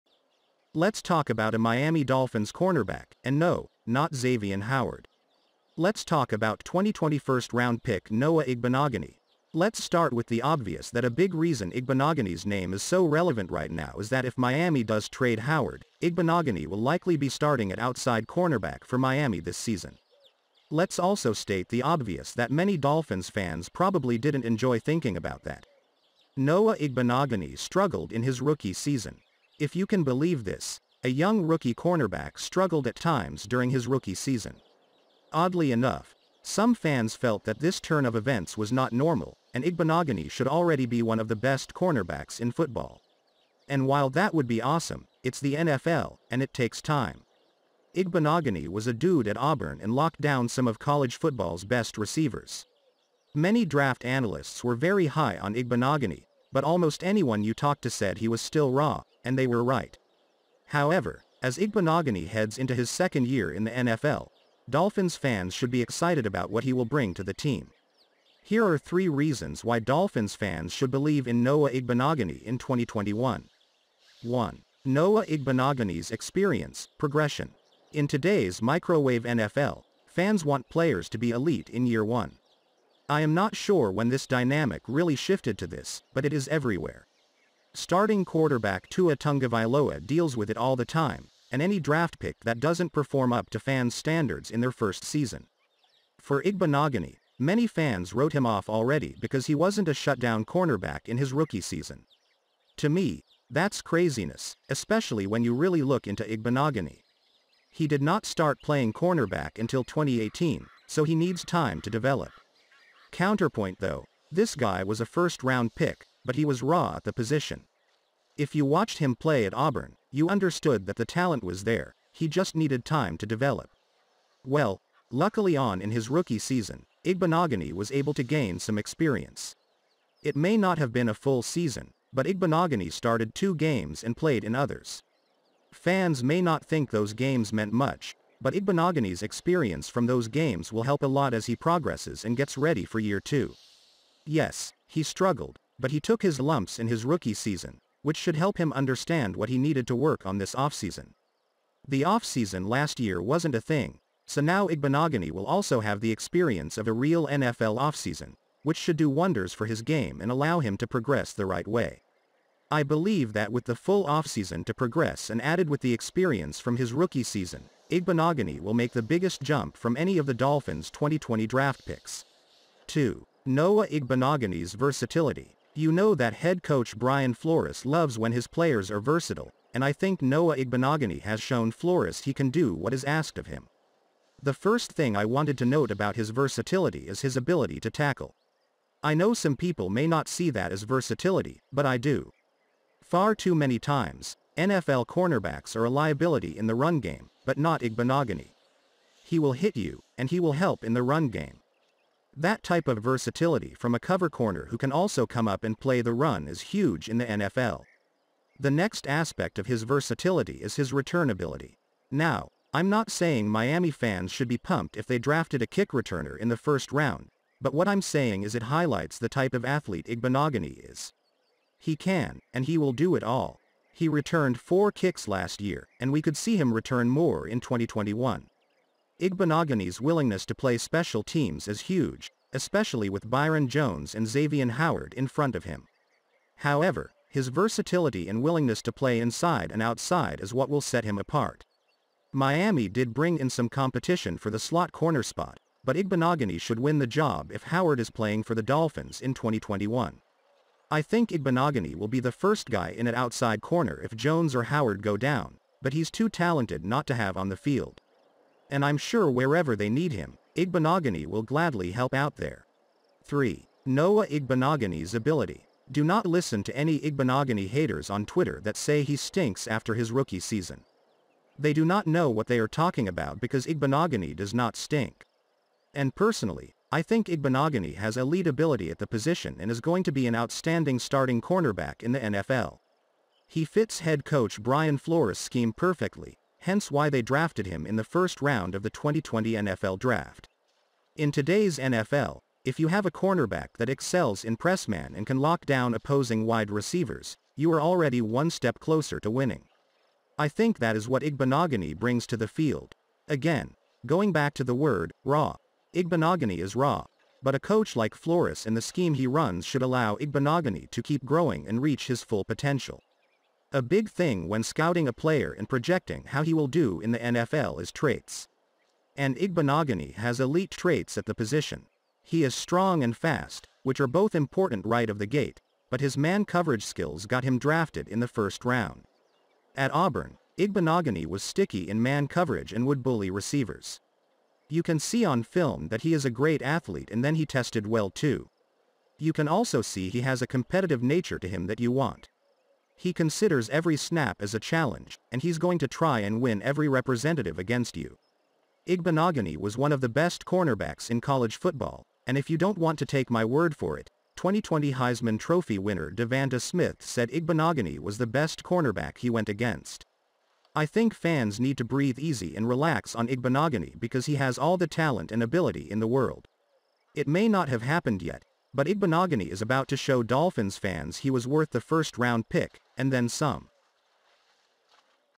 Miami Dolphins GOOD News: "Three" reasons you should believe in Noah Igbinoghene
00:00 3 reasons you should believe in Noah Igbinoghene.
1:14 1. Noah Igbinoghene’s Experience/Progression.
2:52 2. Noah Igbinoghene’s Versatility.
4:23 3. Noah Igbinoghene’s Ability.